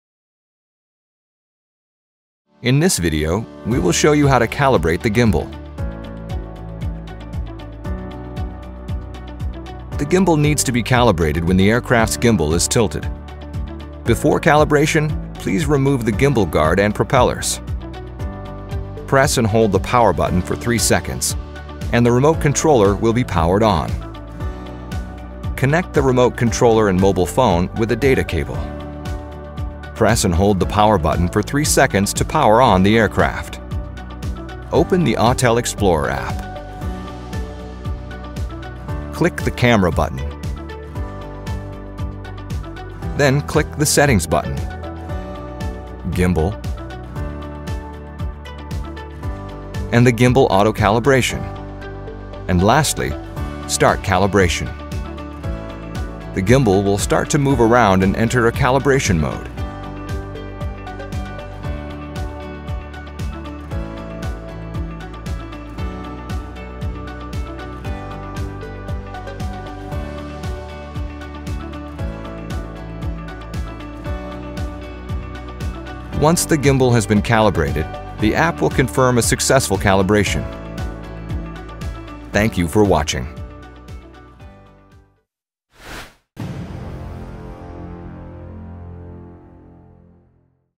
How To Calibrate The Gimbal (Autel EVO II Series)
In this video, we cover the gimbal calibration process in the event the EVO II gimbal becomes skewed.